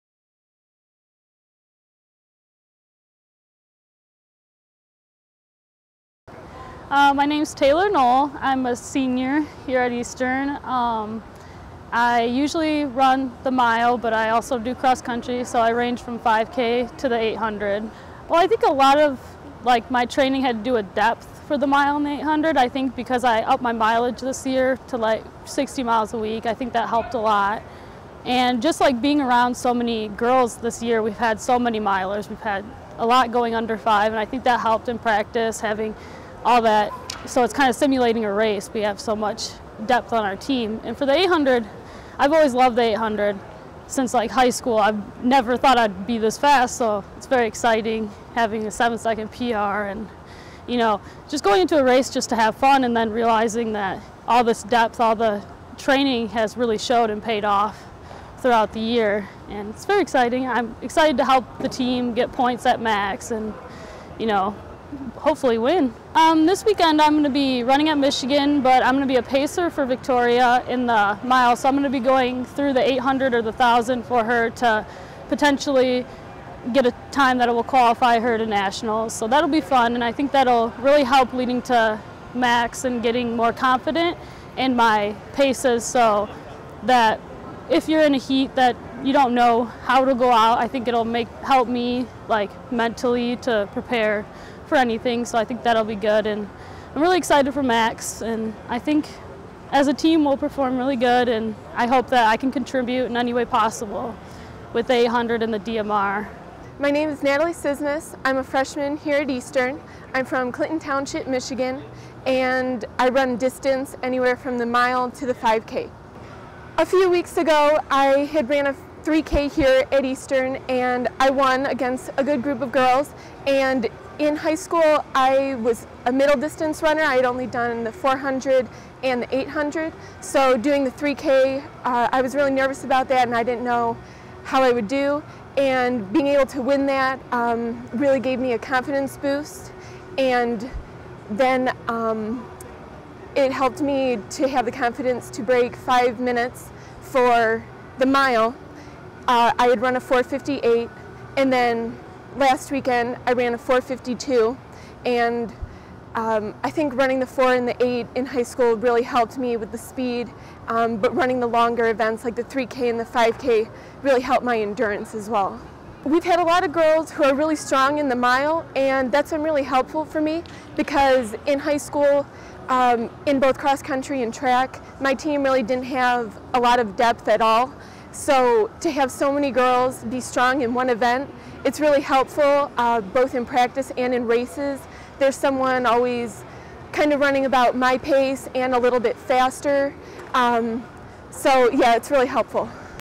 EMU Women's Track and Field Interview Week 6
Senior Taylor Knoll and freshman Natalie Cizmas talk about their marks on the track this season